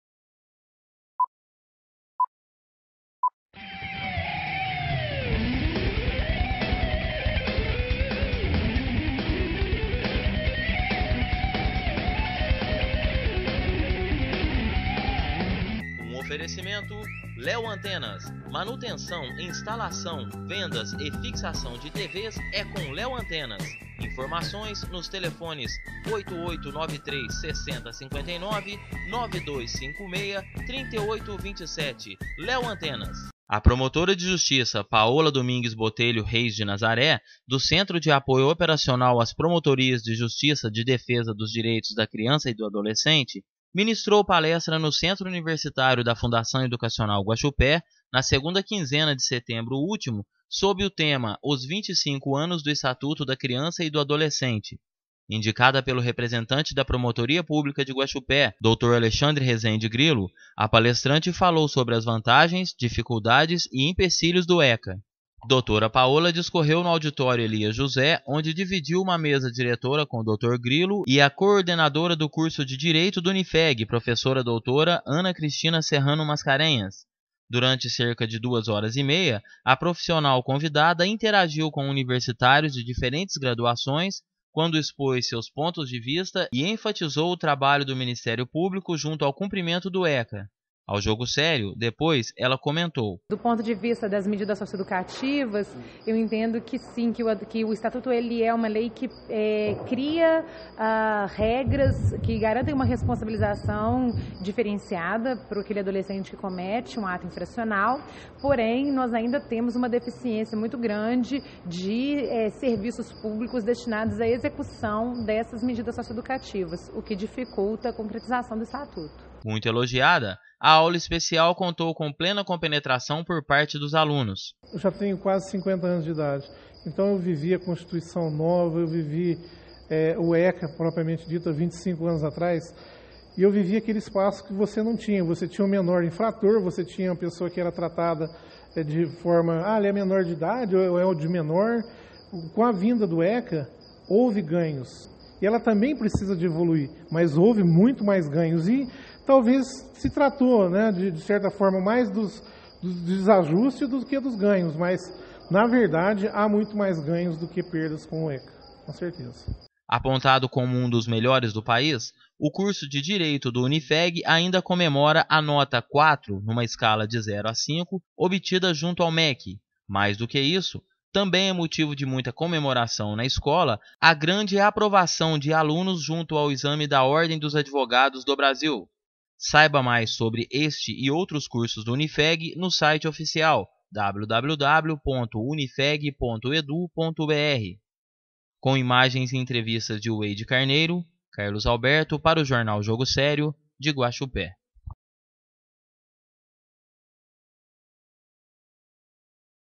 Promotora ministra palestra no Unifeg sobre os 25 anos do ECA
A promotora de Justiça Paola Domingues Botelho Reis de Nazareth, do Centro de Apoio Operacional às Promotorias de Justiça de Defesa dos Direitos da Criança e do Adolescente, ministrou palestra no Centro Universitário da Fundação Educacional Guaxupé, na 2ª quinzena de setembro último, sob o tema "Os vinte e cinco anos do Estatuto da Criança e do Adolescente - ECA". Indicada pelo representante  da Promotoria Pública de Guaxupé, dr. Alexandre Rezende Grillo, a palestrante falou sobre as vantagens, dificuldades e empecilhos do ECA.
Dra. Paola falou no Auditório Elias José, onde dividiu uma mesa diretora com dr. Grillo e a coordenadora do curso de Direito do Unifeg, professora doutora Ana Cristina Serrano Mascarenhas. Durante cerca de duas horas e meia, a profissional convidada interagiu com universitários de diferentes graduações, quando expôs seus pontos de vista e enfatizou o trabalho do MP junto ao cumprimento do ECA. Ao JOGO SÉRIO, depois, ela comentou: "Do ponto de vista das medidas sócio-educativas, entendo que o Estatuto é uma lei que cria regras que garantem uma responsabilização diferenciada àquele adolescente que comete ato infracional. Porém, ainda temos uma deficiência muito grande de serviços públicos destinados à execução destas medidas sócio-educativas, o que dificulta a concretização do Estatuto", disse.
Muito elogiada, a aula especial contou com plena compenetração por parte dos alunos: "A palestra foi muito oportuna, pois é uma forma esclarecedora. Principalmente no Direito, muitas vezes você trabalha com a forma abstrata. Já no caso do ECA, são coisas com as quais a gente convive no dia-a-dia. Eu, que já tenho quase 50 anos, vivi a Constituição nova, vive o ECA, propriamente dito, há 25 anos e vivi naquele espaço em que havia o menor infrator, que era tratado como 'di menor'. Então, com a vinda do ECA, houve ganhos. Não temos mais trabalho infantil, não vemos menores com bebidas em mãos; são evoluções alcançadas com o ECA, que tem mais vantagens do que desvantagens. É lógico que no caso das infrações, o menor comete crime porque fica 'impune'. A lei, então, é como qualquer outra coisa, que precisa de evolução. Mas, na verdade, há muito mais ganhos do que perdas com o ECA", ressaltou o estudante Hércules. CLIQUE AQUI para conhecer mais sobre o Unifeg.